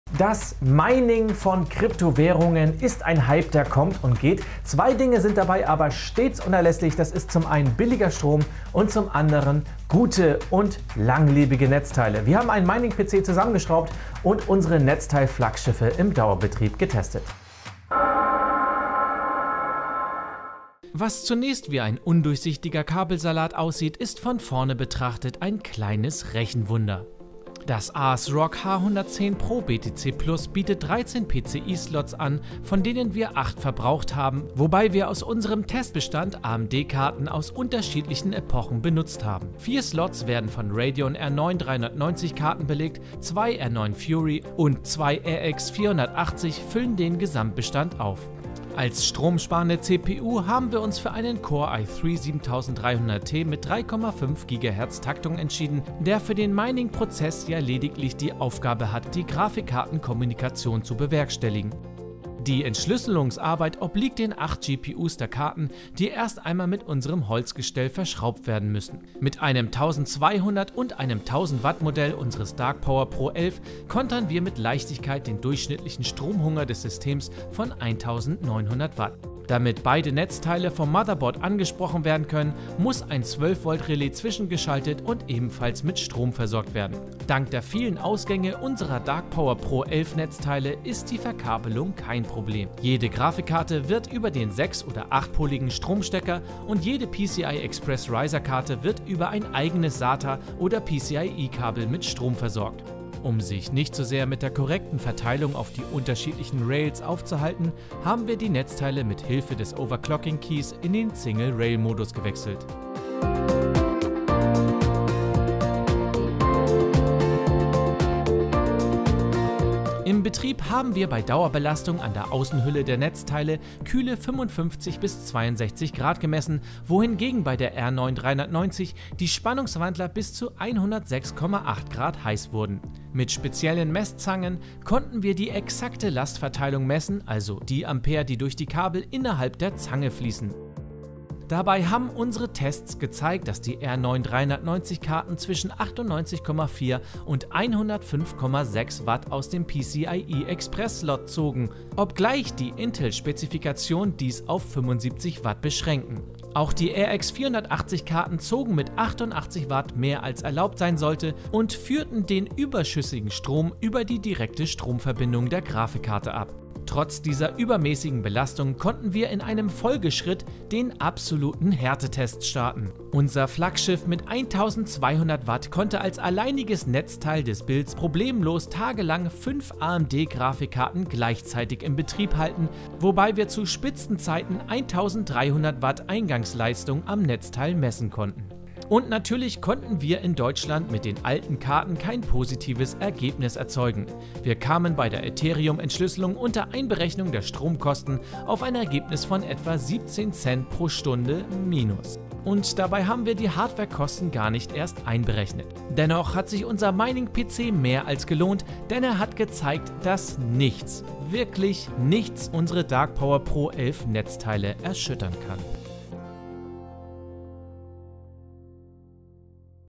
Dark Power Pro 11 im Härtetest: Der be quiet! Mining-PC
Um unser Netzteil-Flaggschiff Dark Power Pro 11 mal so richtig auszutesten, haben wir einen voll bestückten Mining-PC mit acht Grafikkarten gebaut und tagelang Etherium entschlüsseln lassen. . Schaut euch das Video an, um zu sehen, was wir dabei gelernt haben!